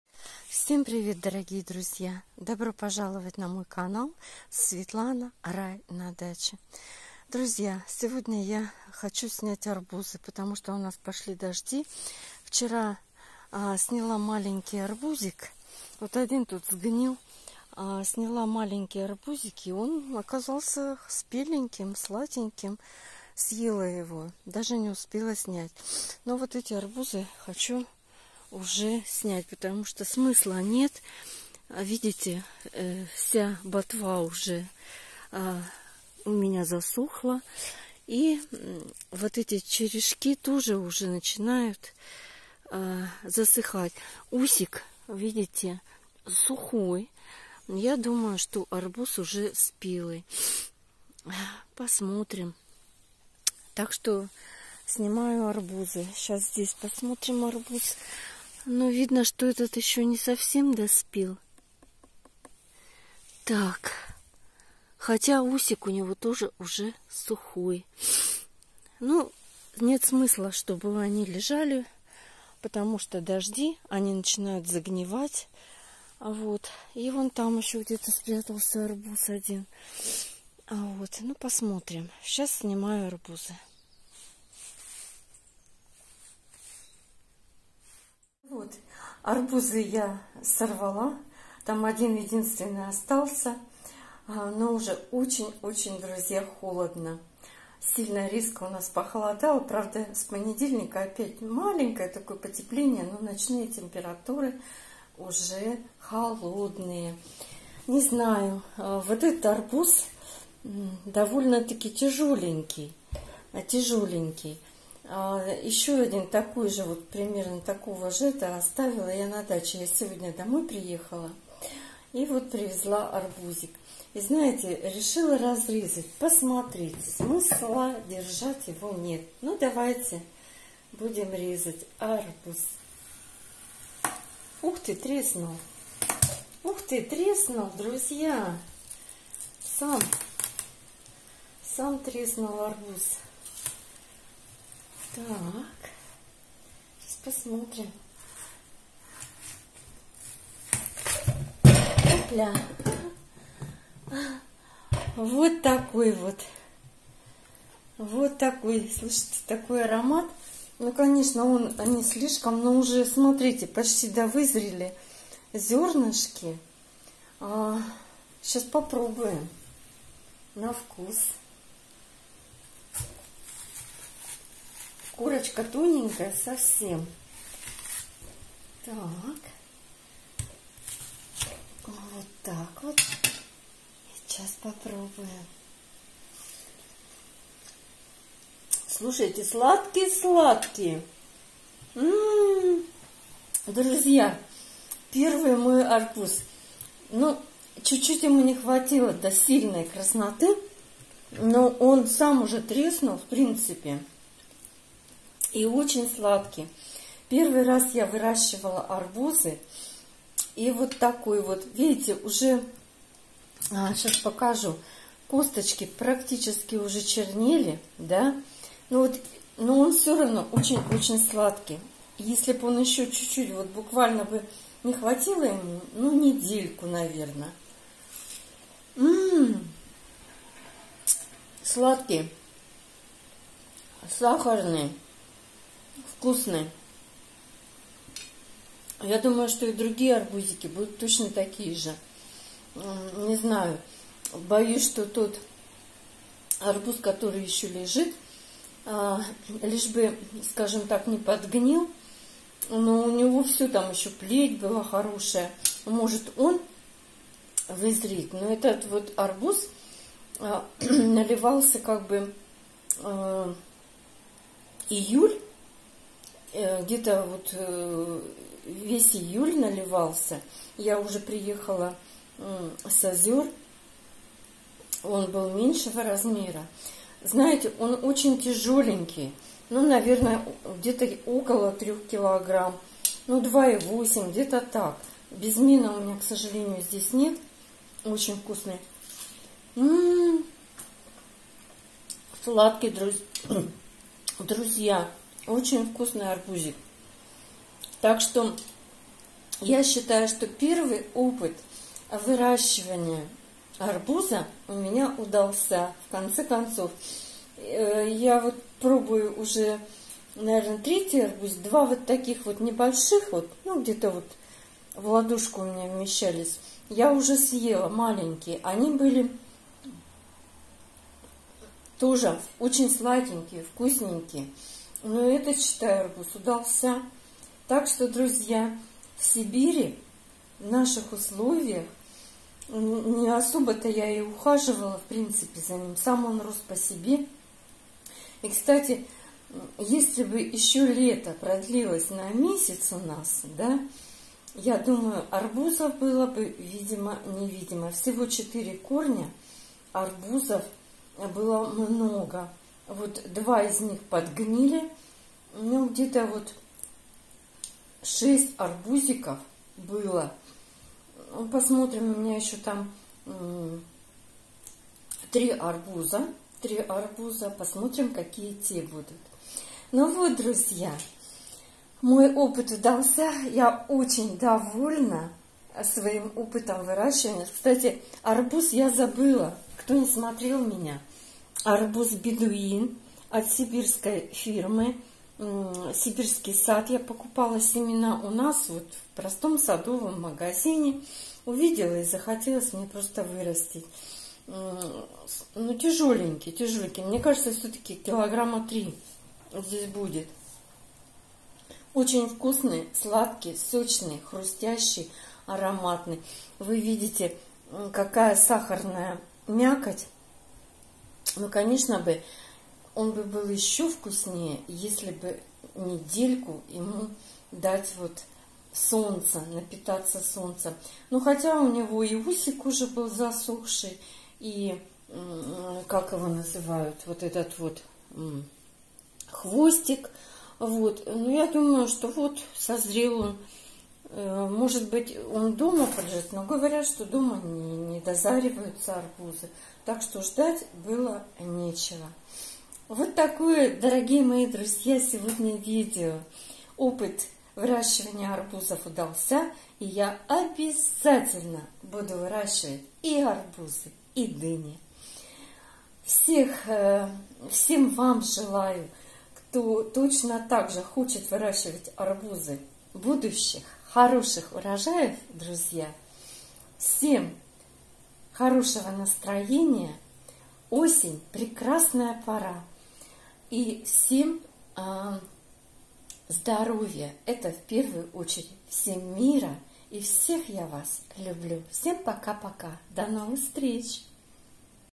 Мой первый УРОЖАЙ АРБУЗА! Снимаю Пробу! Вкусный и сладкий!
Дорогие друзья! Мой первый выращенный арбуз, вкусный и сладкий, очень рада, буду обязательно выращивать!